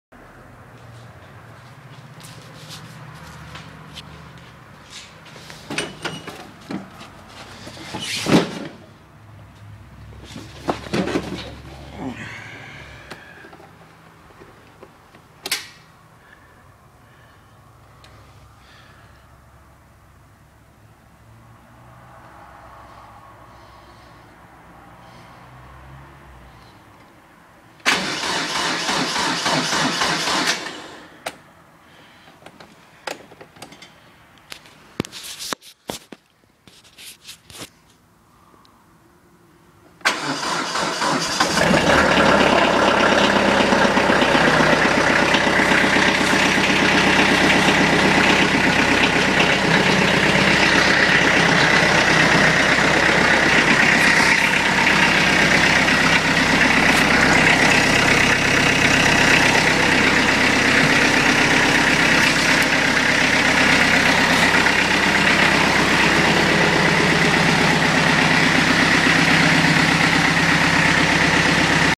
Kubota B3030 1.5 month start!
The first time we started this tractor in a month and a half.